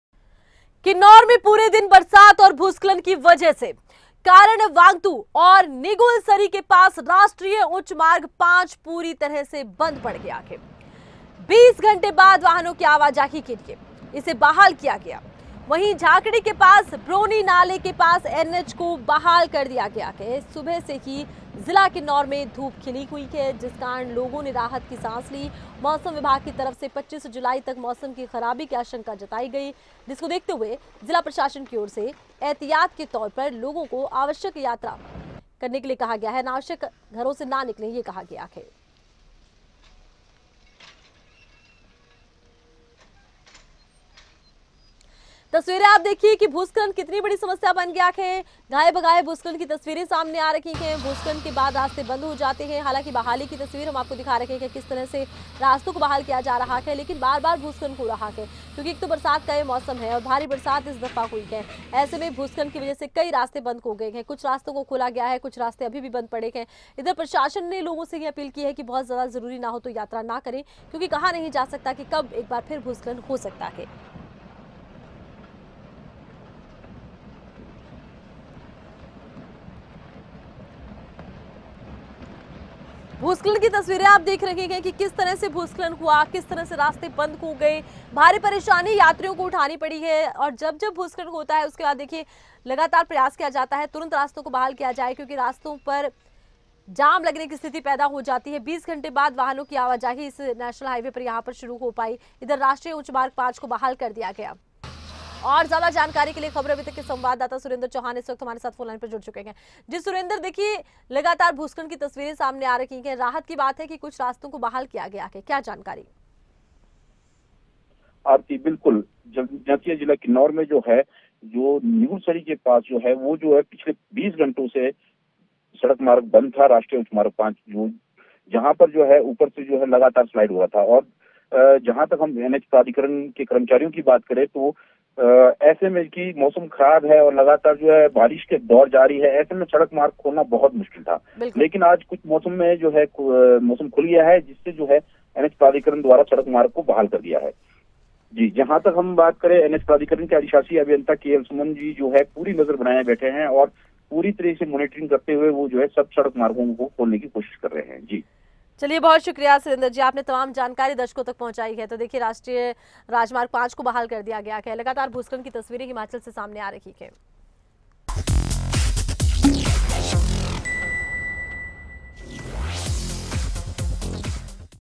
Himachal: 20 घंटे बाद वाहनों की आवाजाही के लिए बहाल हुआ NH-5, लैंडस्लाइड होने से बंद हुआ था मार्ग
Himachal: 20 घंटे बाद वाहनों की आवाजाही के लिए बहाल हुआ NH-5
भारी लैंडस्लाइड होने से बंद हुआ था मार्ग चालकों के लिए बना था परेशानी